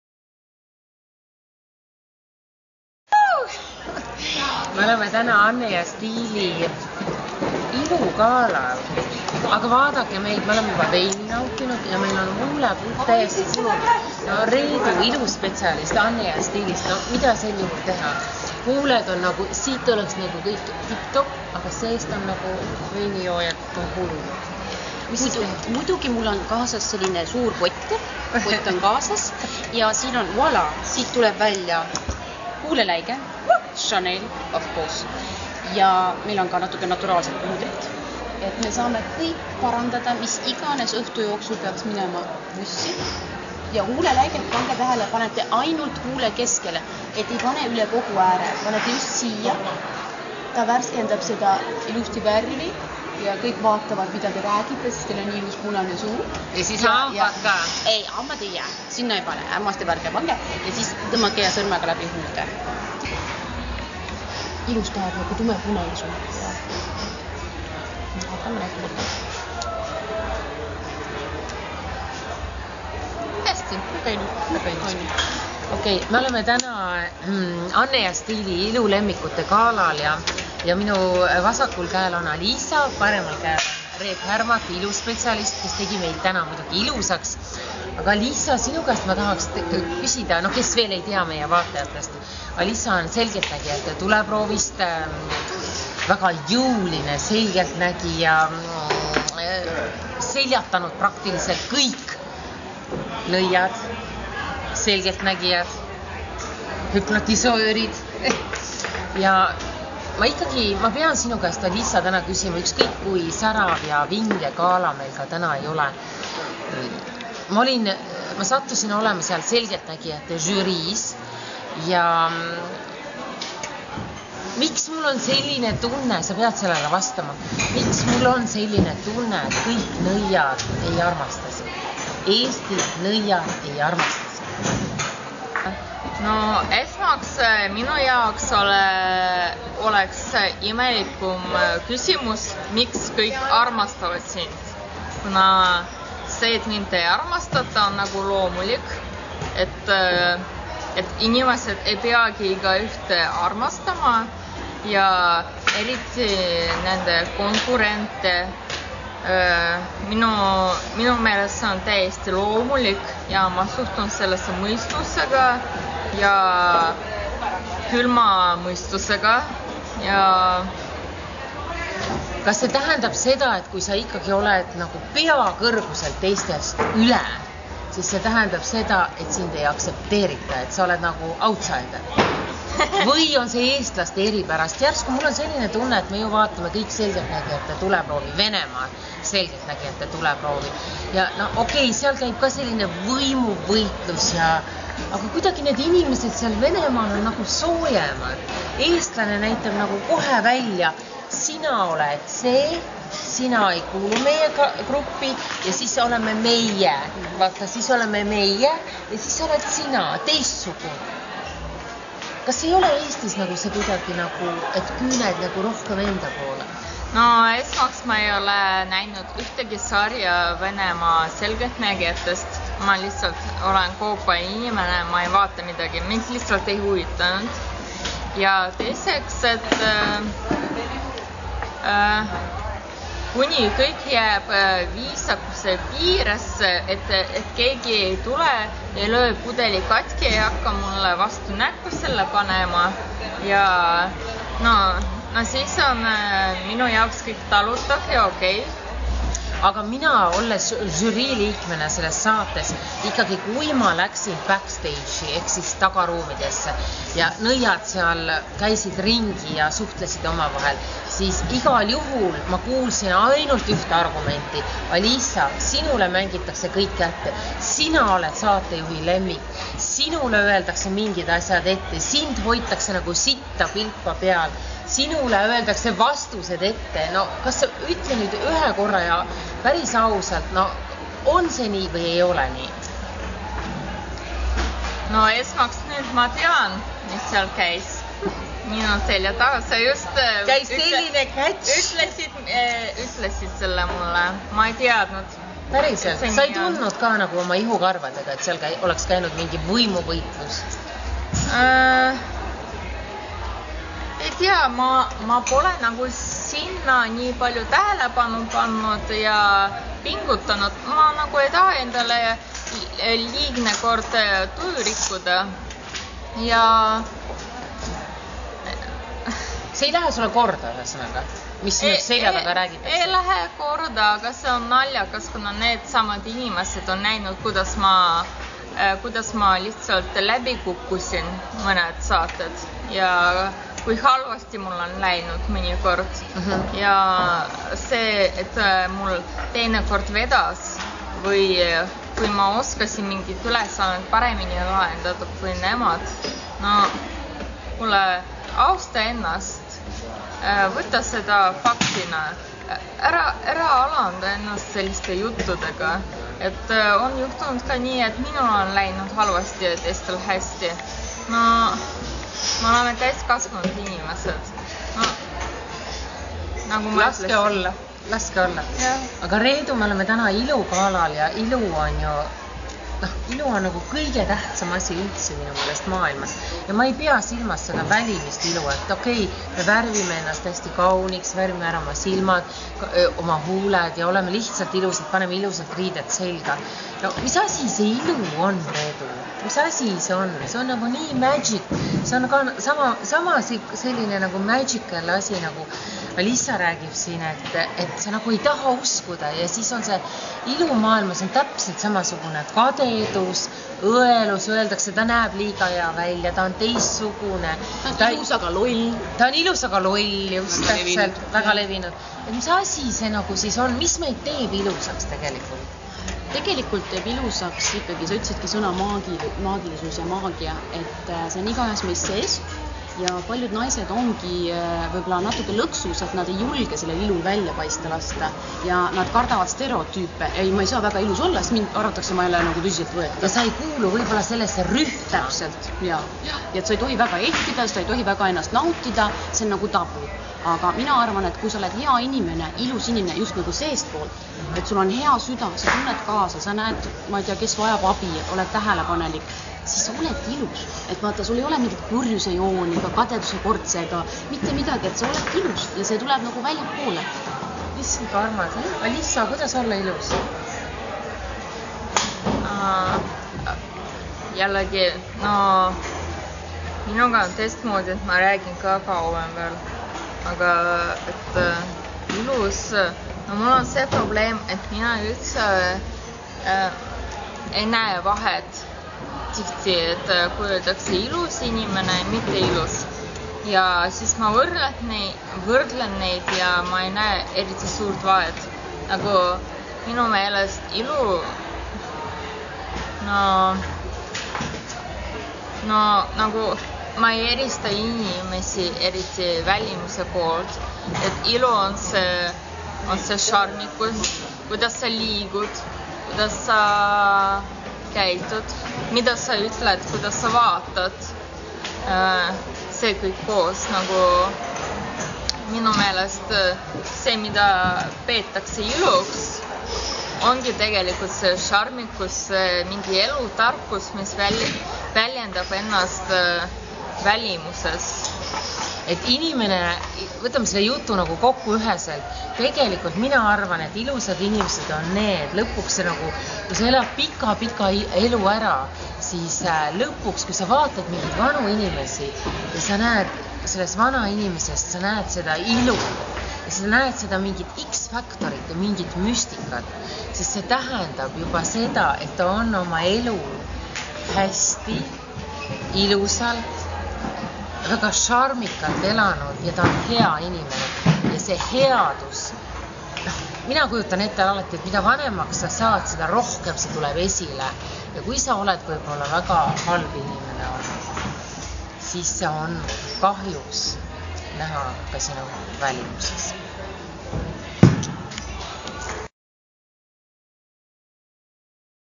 video ohtuleht. ee Anu Saagim & Alisa Verde
Anne & Stiili ilulemmikute gala - kes on kauneim kogu maal?